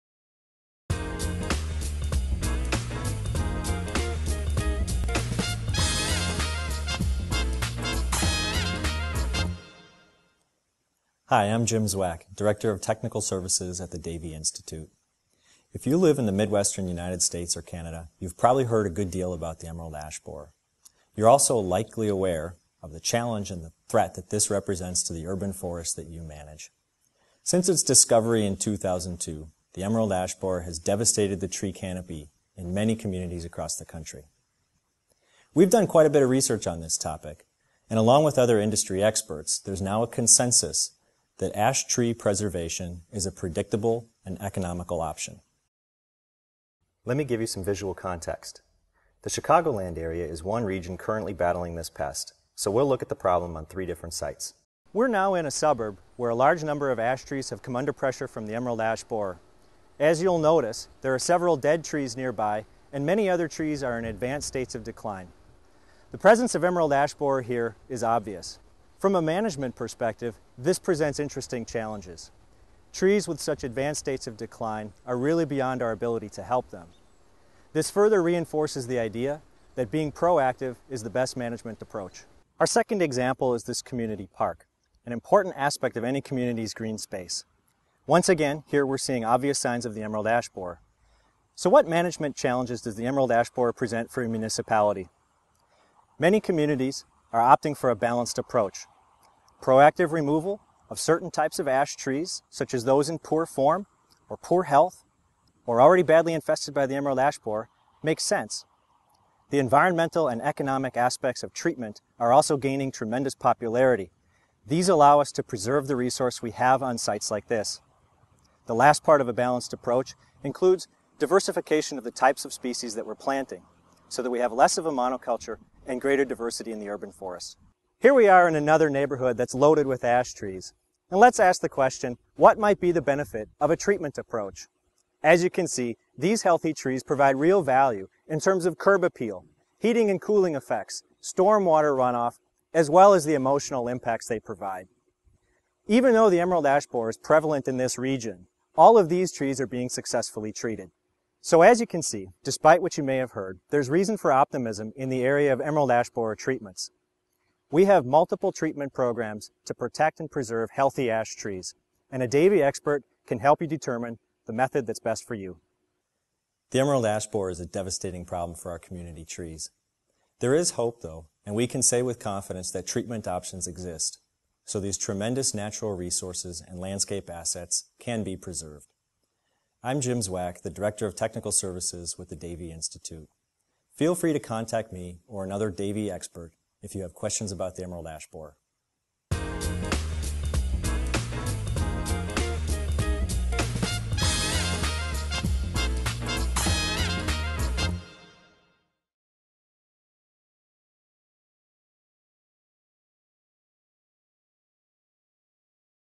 Managing EAB in Your Community: You Have Options!
If you live in the Midwestern US or Canada, you’re probably familiar with Emerald Ash Borer – or EAB and the challenge and threat this tree pest represents to the urban forest that you manage. For more information on managing emerald ash borer,

Jim Zwack, Davey Tree Service expert, discusses the potential impact of EAB on your trees and flexible treatment options as part of a proactive management approach.

Since discovery in 2002, EAB has destroyed millions of ash trees, devastating the tree canopy cover in many communities. Davey Tree and other industry experts have done quite a bit of research and have come to a consensus that ash tree preservation is a predictable and economical option.

Start with identification. Do you have an ash tree and has EAB been spotted in your area? If yes, do your ash trees show signs of already being infested? Inspection is critical. Being proactive is the best management approach.

Here are a few things to watch for:
• D-shaped exit holes and vertical splits in bark
• Canopy dieback from the top down
• Epicormic branches (or sprouts) on the main trunk and major branches that signify the tree is not healthy

What challenges does EAB present for municipalities? Many communities are opting for a balanced approach. Proactive removal of certain types of ash trees, such as those that are in poor health or are already infested by EAB, makes sense. Also consider diversification of the types of tree species that are planted so that there is less of a monoculture in the urban forest.

Despite what you may have heard, there is reason for optimism in the area of EAB treatments. We have multiple treatment programs to protect and preserve healthy ash trees.

Contact your local certified arborist to help you determine the EAB treatment method that’s best for you.